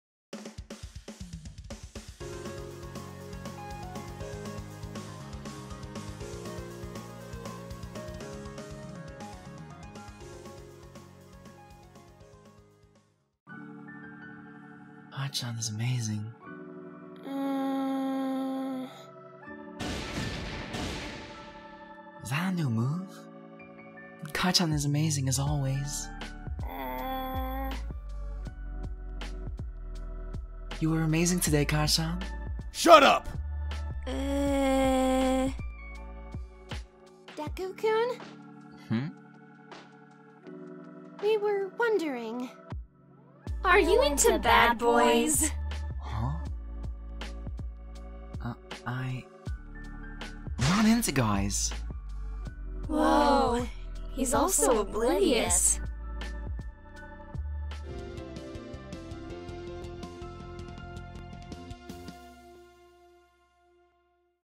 Is Deku into bad boys? || MY HERO ACADEMIA COMIC DUB [VA ROOKIES]
VOICES:
— Deku: RenNya VA
— Bakugou: Cave
— Uraraka: persephiia
— Tsuyu: Anvitha Amuluru
— Mina: Cronch

Hello! My name is Harudori, but you can call me Phebe! I'm a weeb
who has officially decided to start the performing arts life!